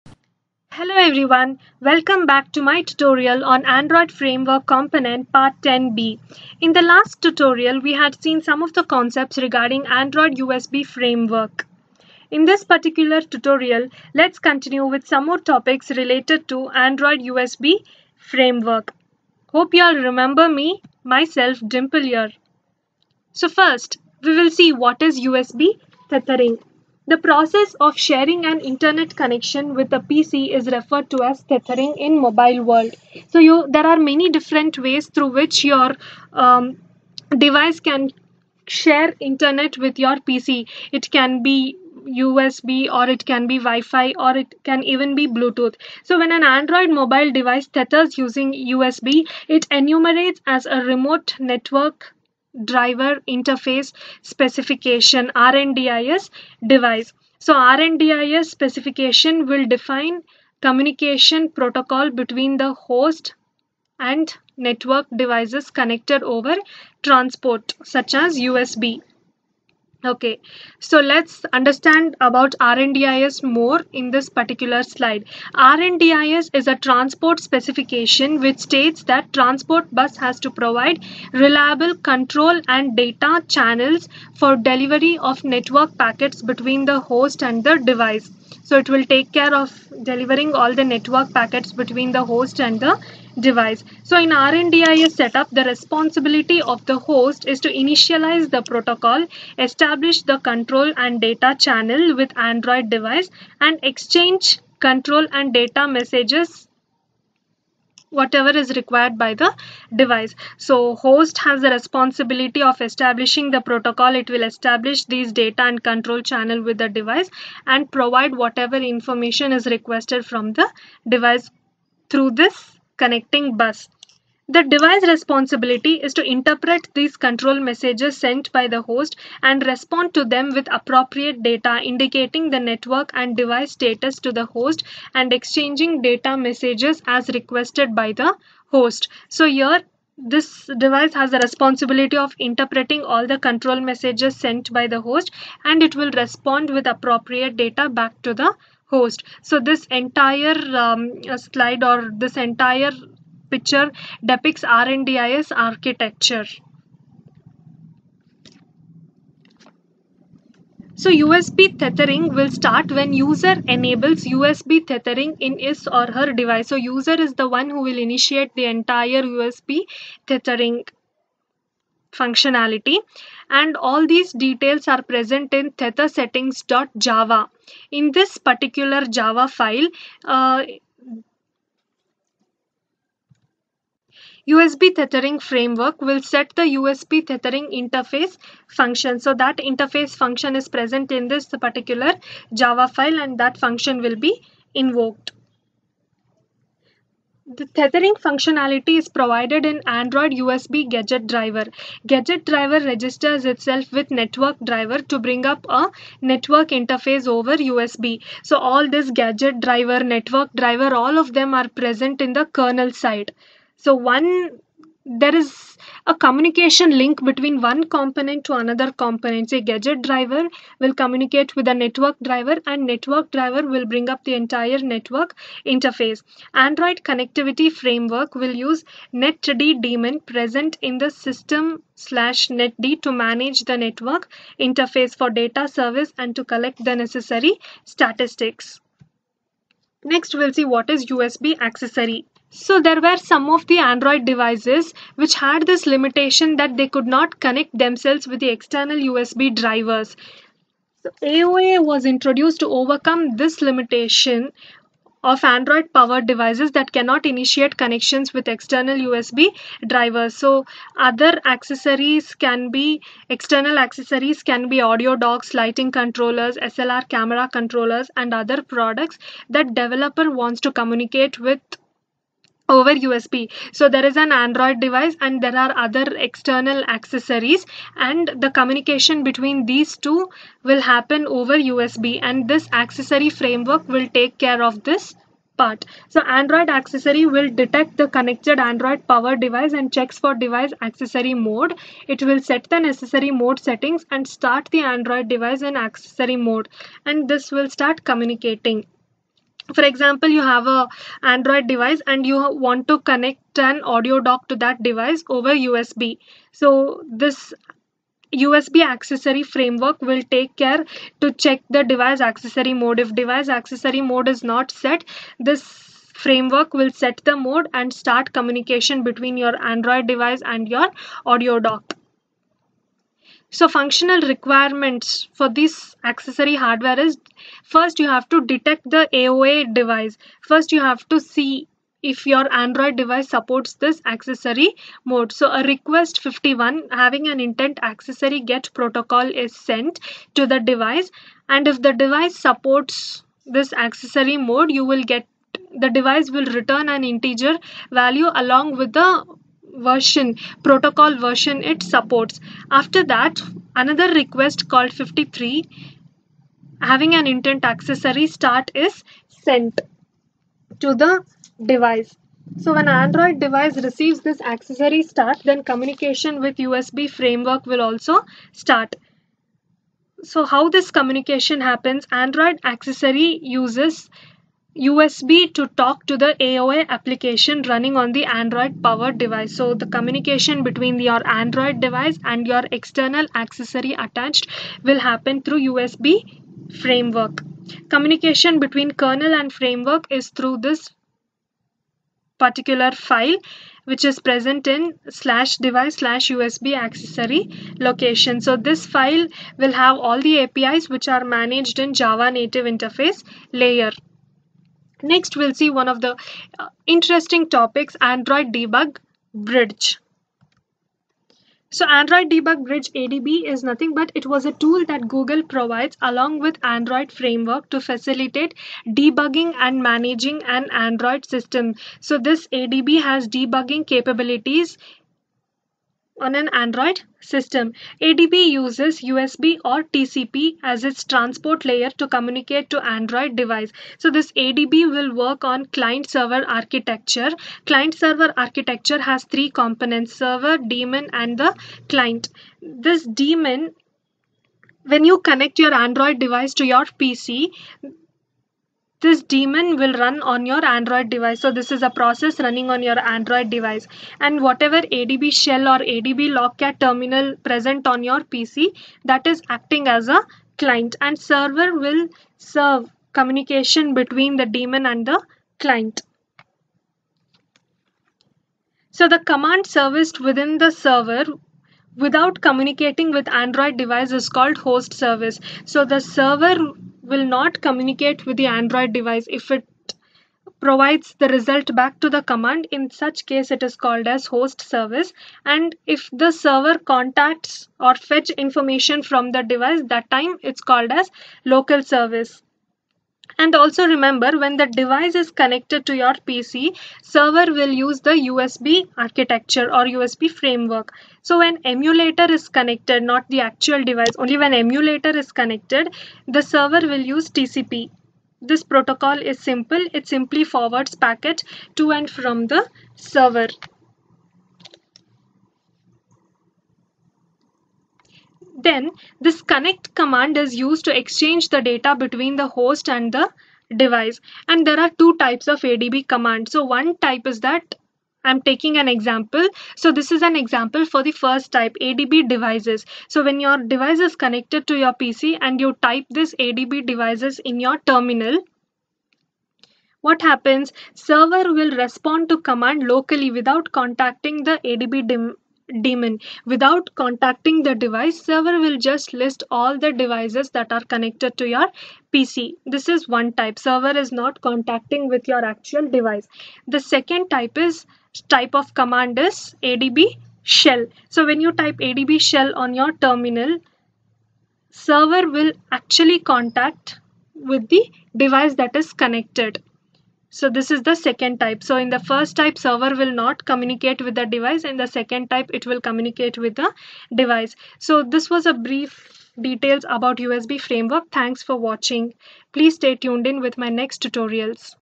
Android Framework- USB Manager-part2
This  continued part of the tutorial will provide some more additional details on Android USB Framework Manager which includes USB Tethering , USB Accessory  and ADB  .